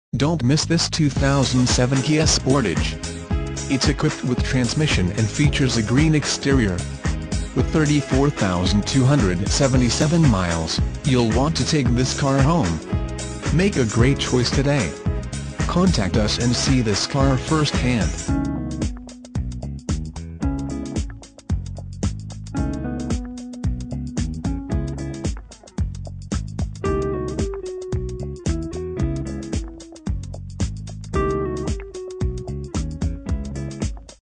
2007 KIA SPORTAGE Council Bluffs, IA T7378B
2007 KIA SPORTAGE Council Bluffs, IA
Stock

1010 34th Ave.

Don't miss this 2007 Kia Sportage. It's equipped with  transmission and features a GREEN exterior. With 34277 miles, you'll want to take this car home. Make a great choice today.